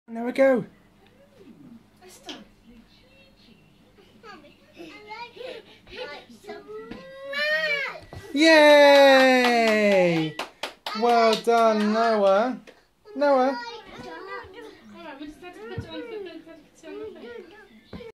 Noah's first steps
Noah finally taking his first steps